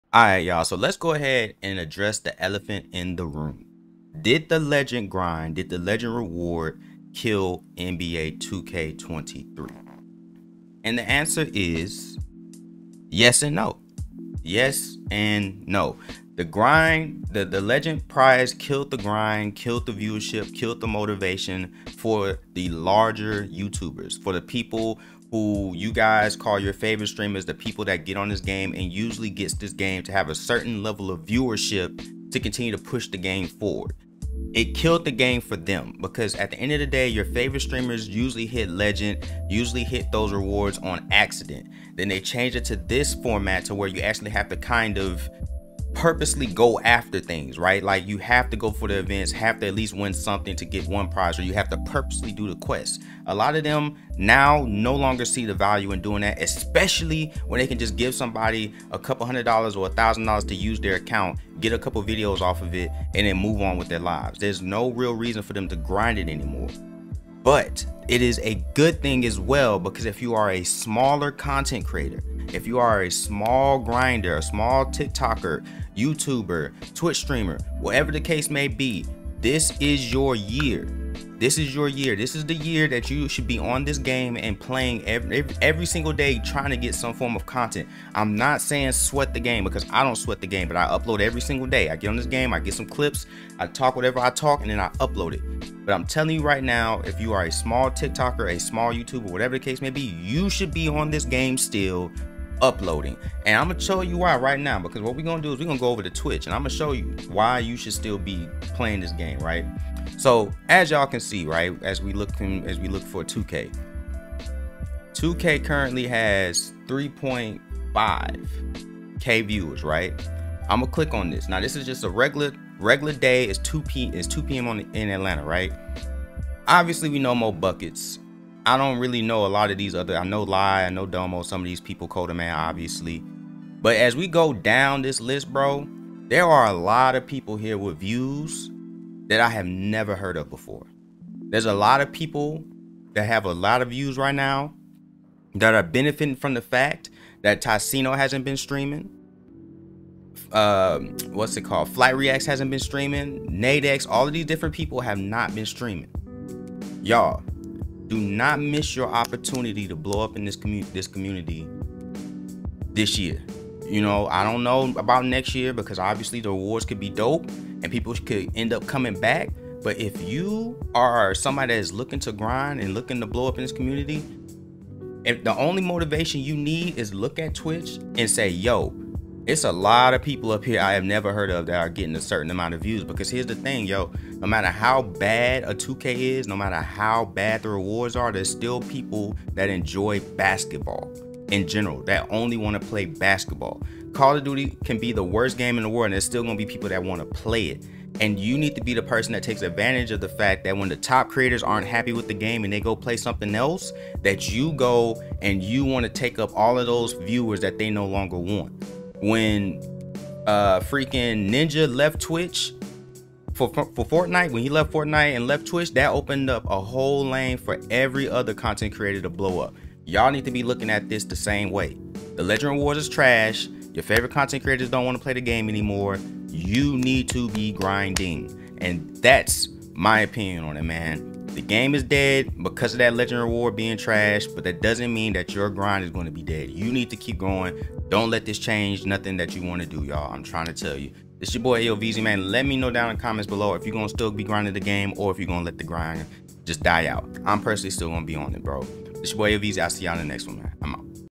Why The NBA 2k23 Legend Reward Being BAD Is a GOOD THING!
with the nba 2k23 legend reward being lack luster heres a reason to continue to play the game and continue to put out content so that you can receive your logo!